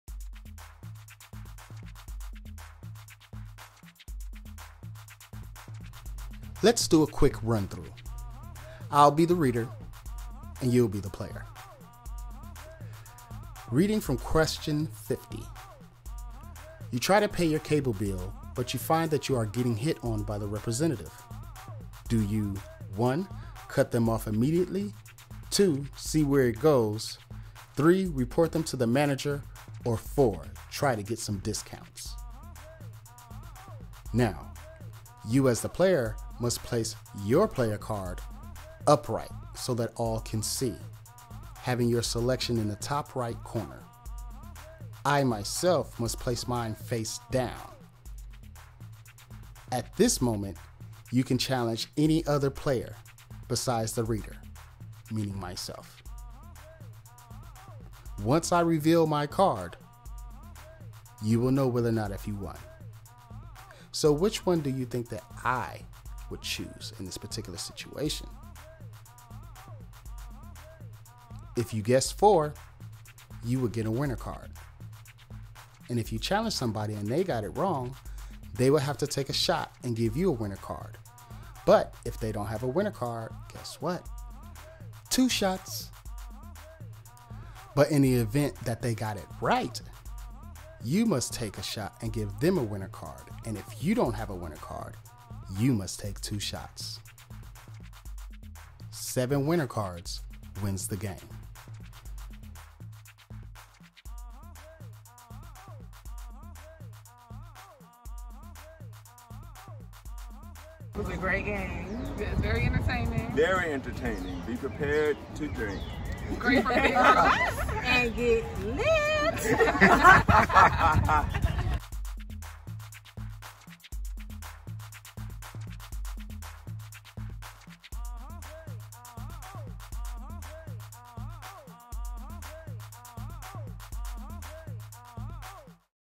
How to play "WWTD? What Would They Do?
This is a video to teach you how to play WWTD? What Would They Do?  Now available on

and Amazon